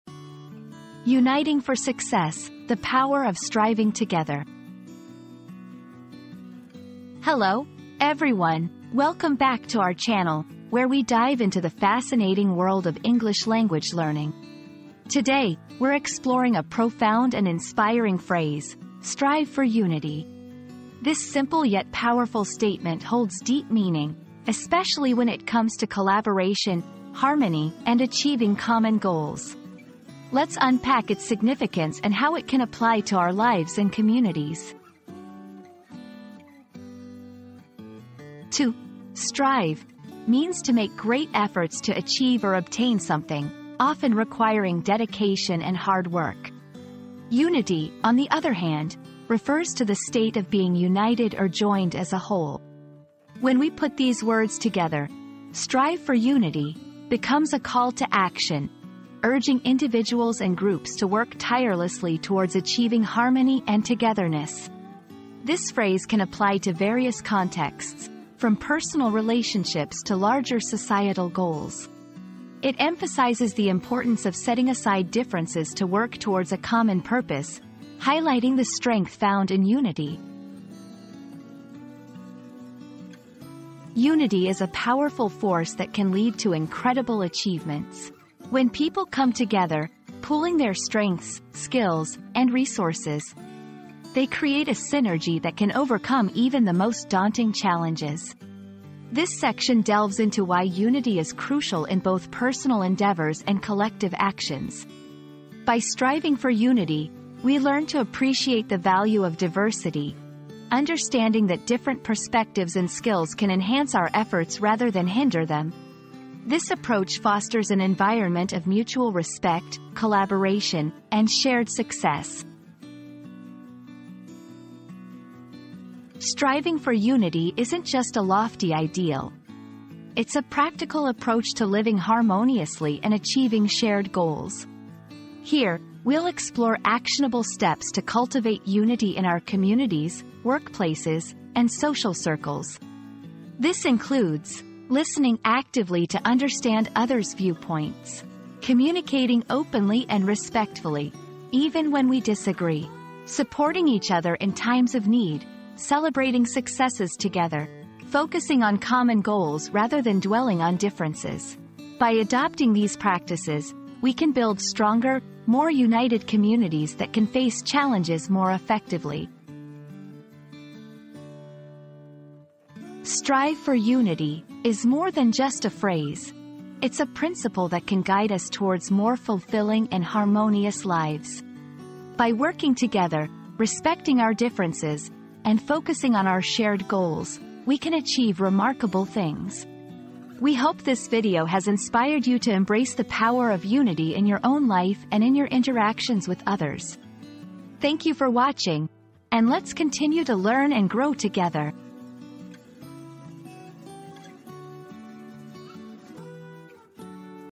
Uniting for Success: The Power of Striving Together
00:00 • Introduction - Uniting for Success: The Power of Striving Together
00:37 • Understanding "Strive for Unity"
01:23 • The Importance of Unity in Achieving Goals
02:05 • How to Strive for Unity in Everyday Life